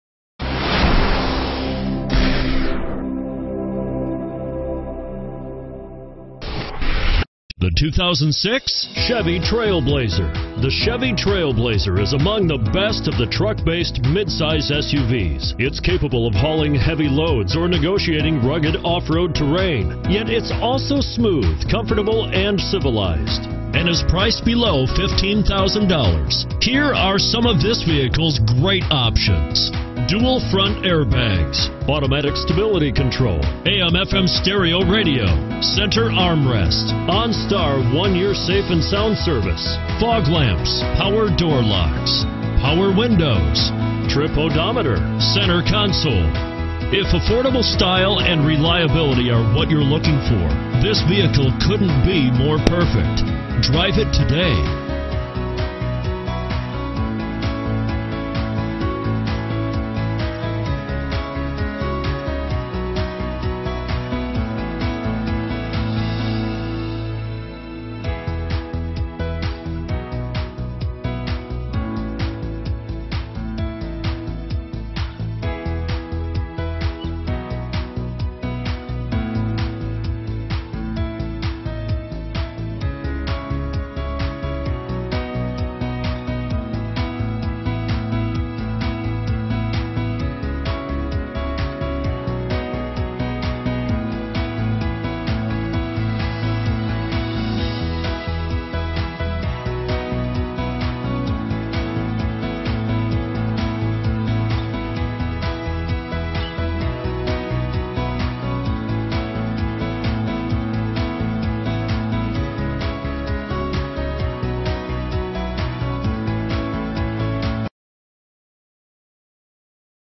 Chevy Trailblazer Wolfforth TX P04326
2006 Chevrolet TrailBlazer 4DR 2WD EXT LS

With loads of interior room, a long list of features, and easily accessible seating, this 2006 TrailBlazer makes it to the top of the list of attractive family vehicles! Our Rear Wheel Drive comes with incredible power from the strong 4.2L V6 engine kicking out 291hp and earning 22mpg highway. Our gallant SUV is matched perfectly to a 4-Speed automatic transmission and the Premium Smooth Ride suspension package for an ultra soft highway ride. Our LS trim is immaculate, and the value just can't be beat. It's quick, fun to drive around town, comfortable to ride in, and capable of dragging your boat behind you. This LS is full of features to make your drive enjoyable. Plenty of power accessories, Premium cloth upholstery, OnStar, a nice sound system, ABS, and automatic headlights give you everything you want and need in a family cruiser with a price point that the rivals can't come close to. Stop in today and surprise yourself. You will be glad you did!